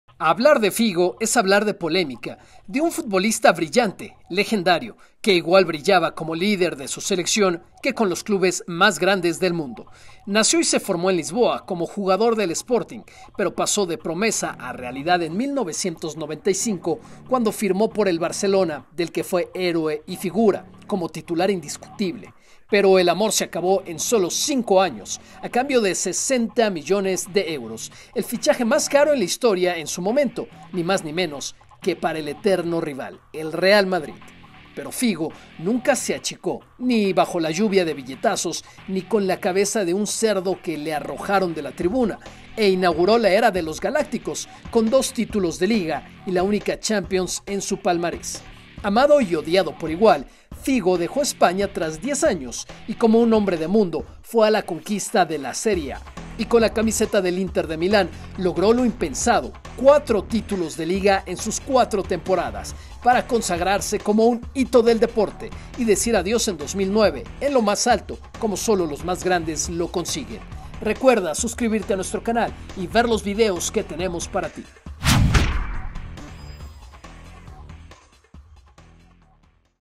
Luis Figo, el crack de la polémica
No hay forma de poner en duda la calidad de Luis Figo y tampoco su temple de acero, pues en gran parte de su carrera estuvo envuelto en polémica, sobre todo con el cambio del Barcelona al Real Madrid y se mantuvo firme, cumpliendo dentro de la cancha... ¡Todo un crack!